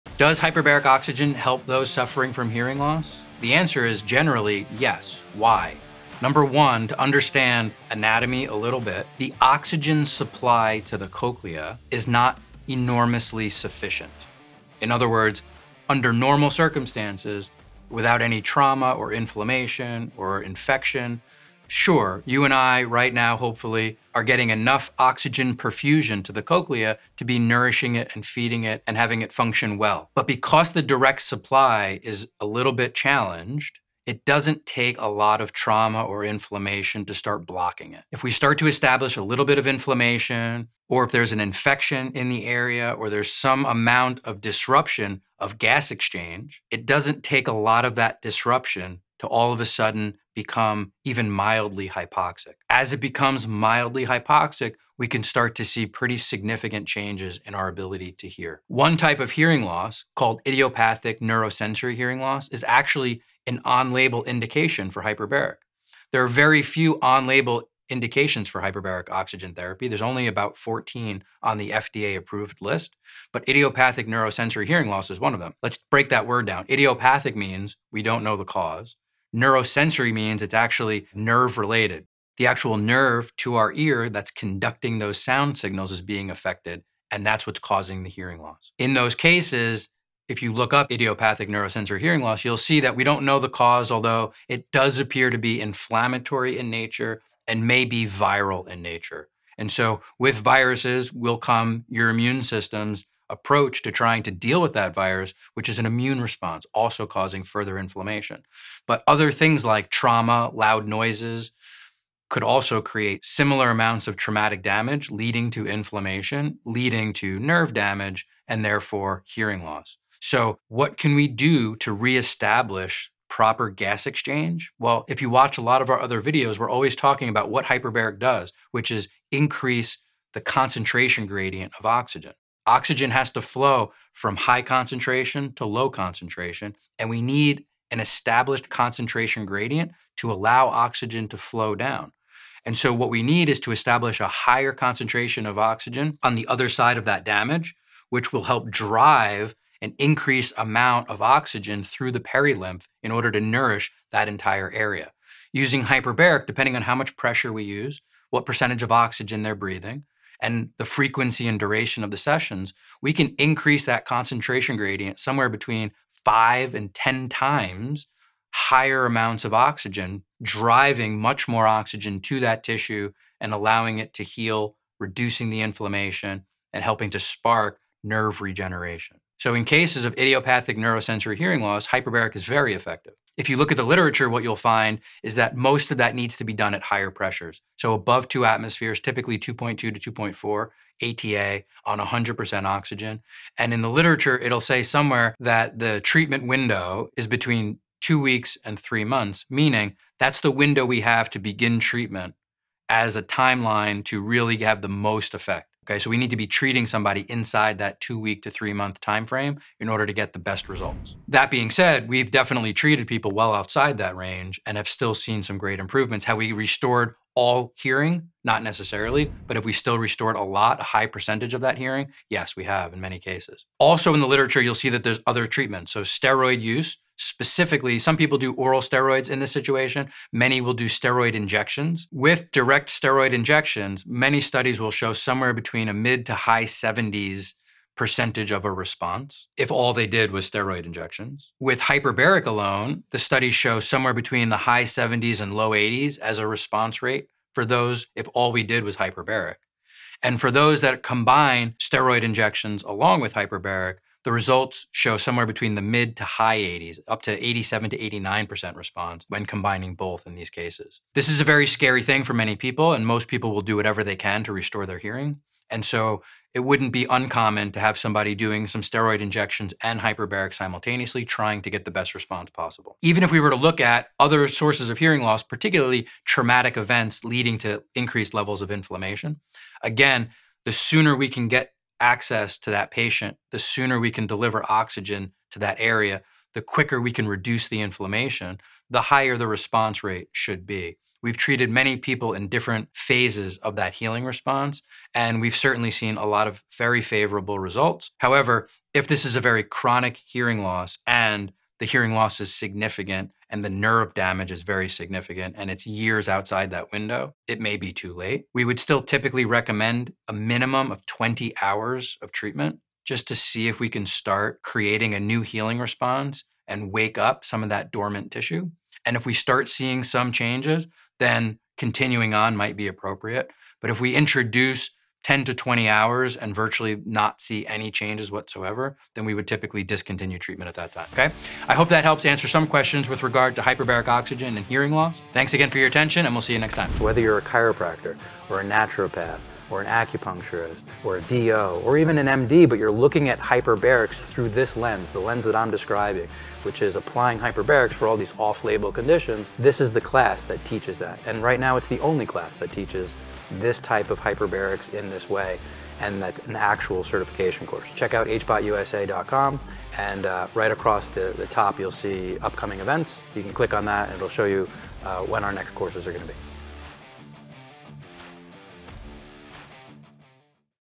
Does Hyperbaric Oxygen Therapy Help With Hearing Loss?
Can you use HBOT for hearing loss? Watch this video to find out!

Losing your hearing is scary. If you, a loved one, or a patient of yours has experienced hearing loss, you may be looking for any and all possible treatments to recover as much hearing as possible.

Perhaps you’re wondering if the hyperbaric chamber can help a person who’s suffering from hearing loss.

The short answer is…maybe! As usual, it depends on a number of factors. But generally speaking, HBOT is likely to help. And as usual, the sooner you can get that person in the chamber, the better the response is likely to be.

Join me as we discuss how hyperbaric oxygen therapy may be able to restore hearing loss.

STUDIES:

Hyperbaric Oxygen Therapy for Sudden Sensorineural Hearing Loss after Failure from Oral and Intratympanic Corticosteroid